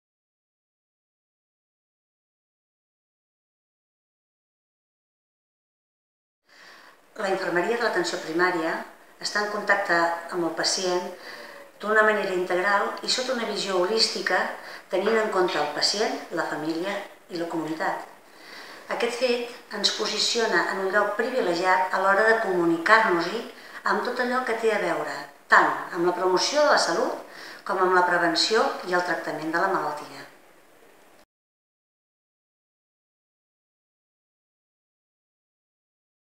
Dolors Pons, de l'EAP Baix Berguedà, destaca que la infermeria aborda el pacient de manera integral
Amb motiu de la celebració del Dia Internacional de la Infermera, el 12 de maig, la infermera de l'EAP Baix Berguedà, Dolors Ponts, explica que la infermeria està en contacte amb el pacient de manera integral.